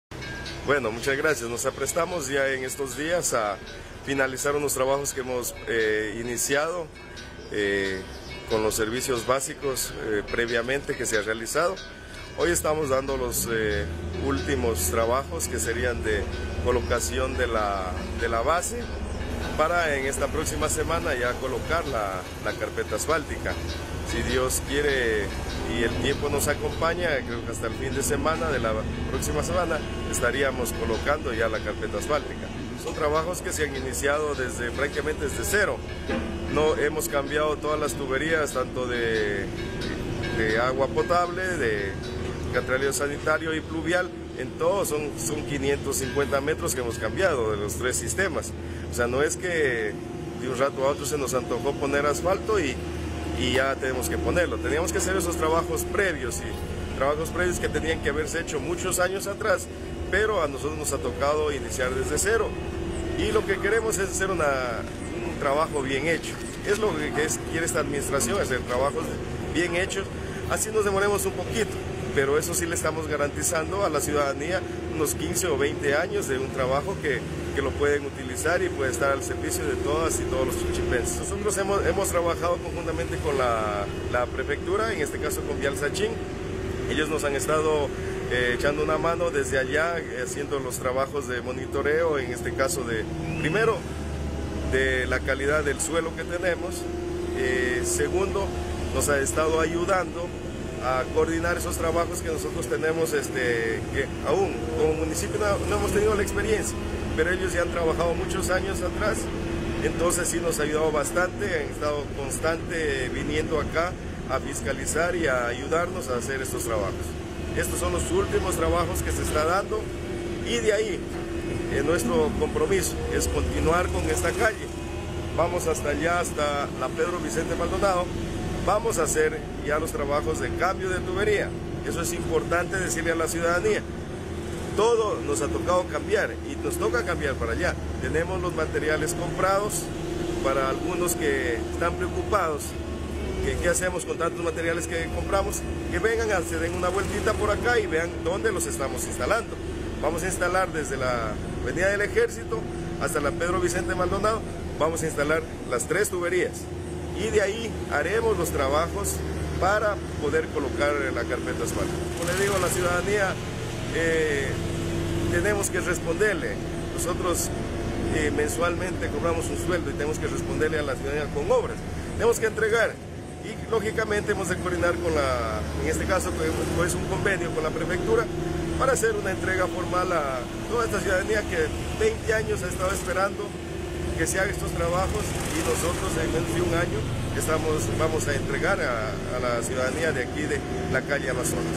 El Municipio de Chinchipe Avanza con Obras de Infraestructura Vial
Chinchipe, Ecuador, 26 de mayo de 2024 - El Municipio de Chinchipe, en coordinación con el Consejo Provincial, está concluyendo los trabajos de colocación de la base para iniciar con la colocación de la carpeta asfáltica en la calle Amazonas. El alcalde Henry Ordóñez brindó detalles sobre estas labores en curso.